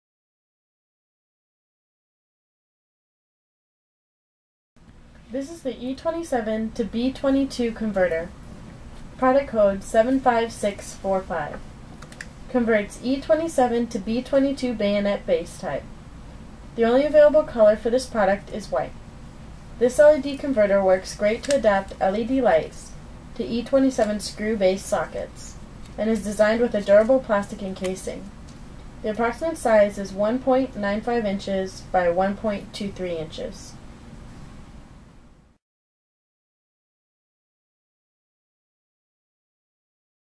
75645 E27 To B22 Converter
E27 To B22 Converter, product code: 75645. Converts E27 to B22 bayonet base type. Only available color is white. Converter works great to adapt LED Lights to E27 screw base sockets and is designed with a durable plastic incasing.